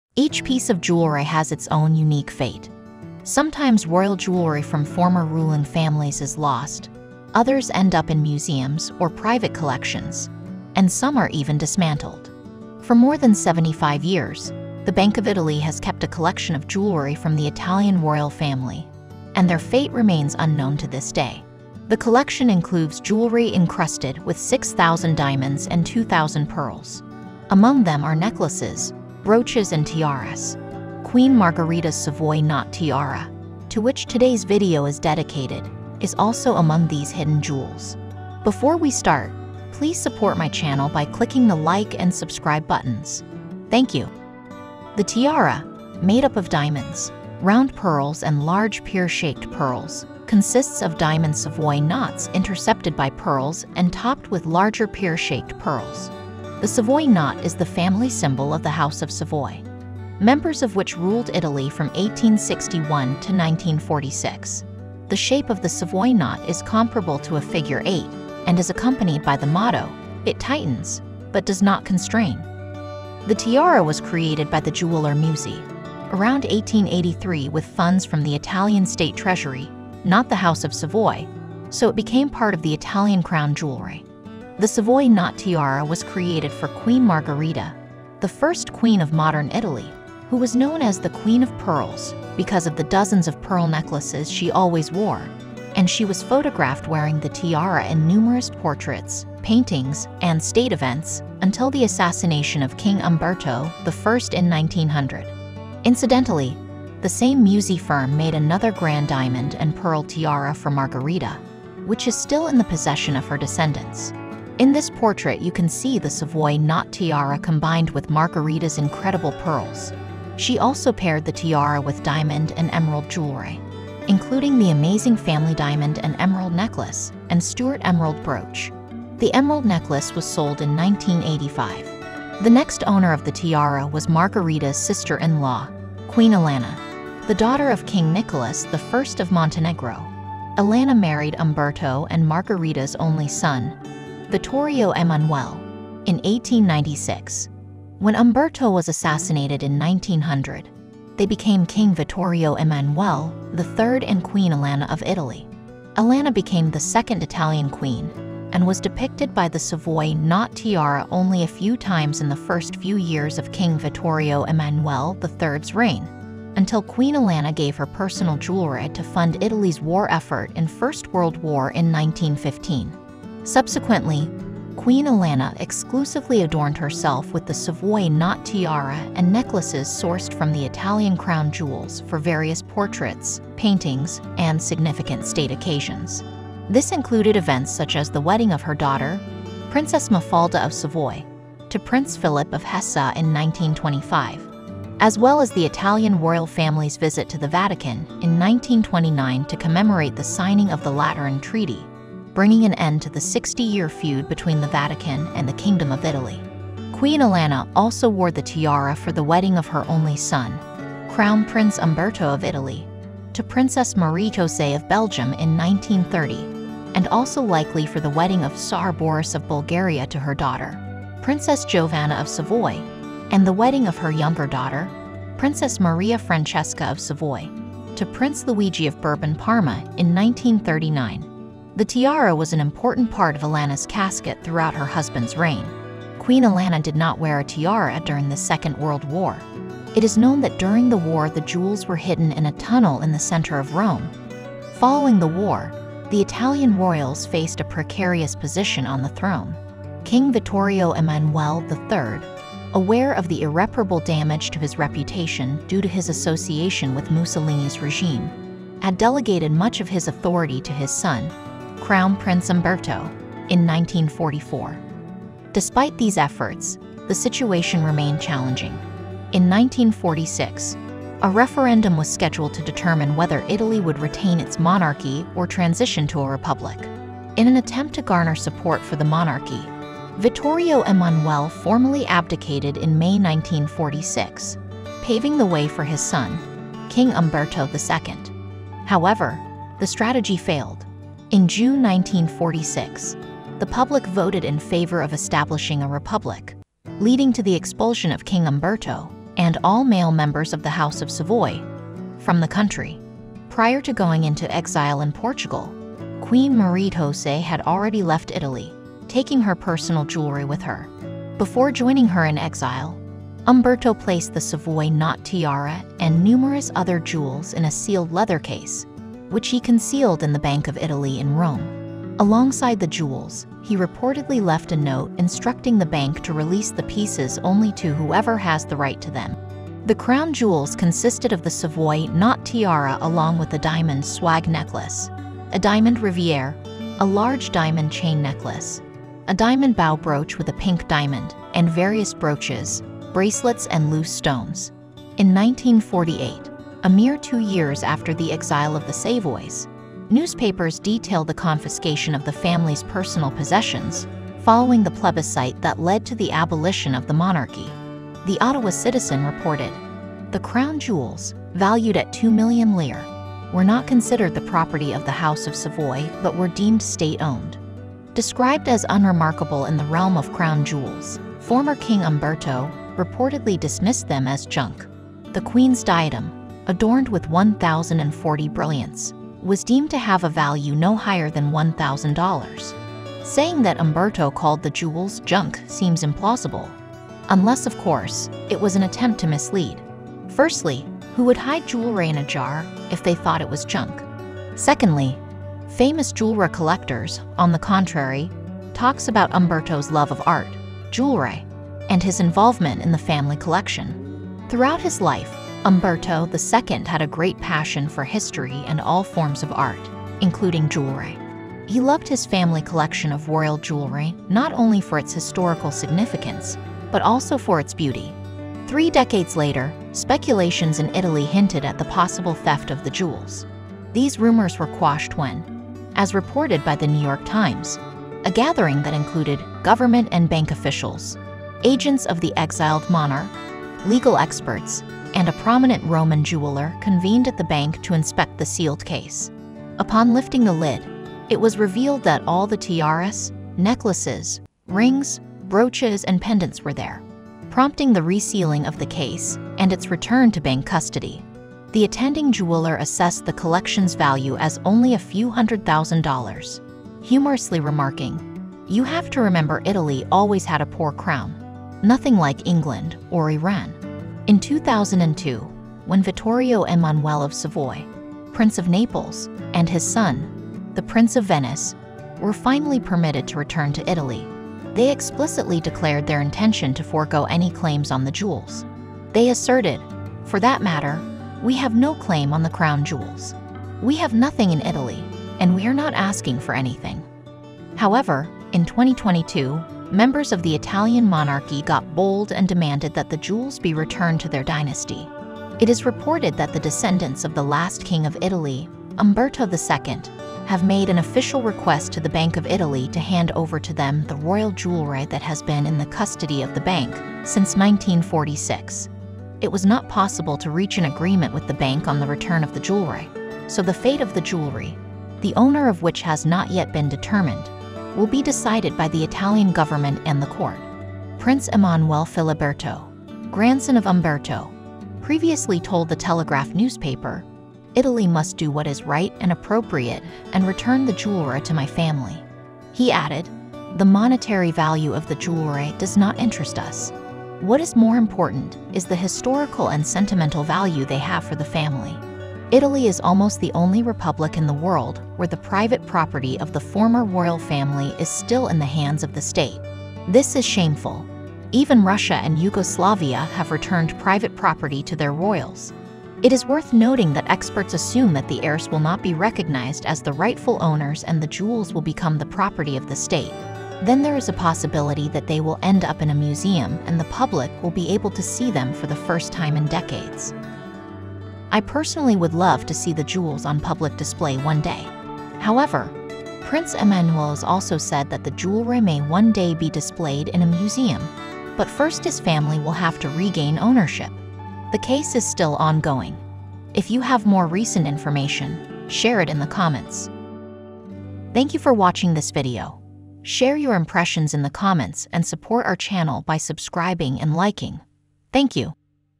Queen Margherita’s Savoy Knot Tiara: Tiara wanted by Italy's former royal family
Each piece of jewellery has its own unique fate. Sometimes royal jewellery from former ruling families is lost, others end up in museums or private collections, and some are even dismantled. For more than 75 years, the Bank of Italy has kept a collection of jewellery from the Italian royal family, and their fate remains unknown to this day.
The collection includes jewellery encrusted with 6 thousand diamonds and 2 thousand pearls. Among them are necklaces, brooches and tiaras. Queen Margherita's Savoy Knot Tiara, to which today's video is dedicated, is also among these hidden jewels.